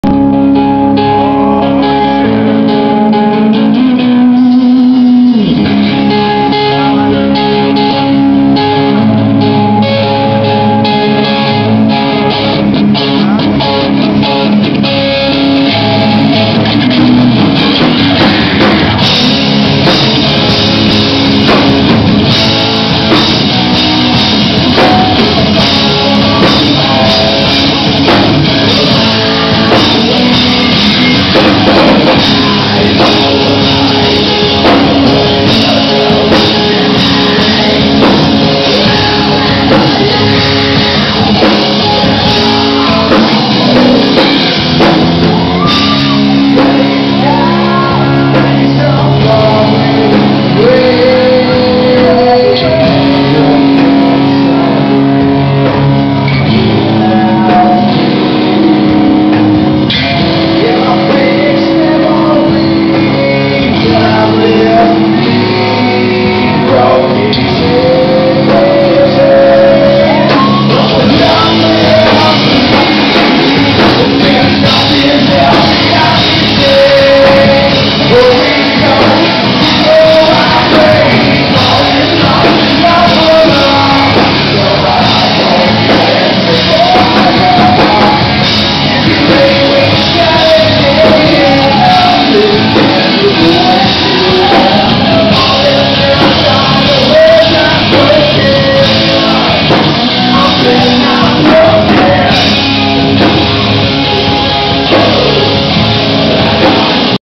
Dopemine Redmond Fire House 5/23
Local Seattle band Dopemine at the Redmond teen center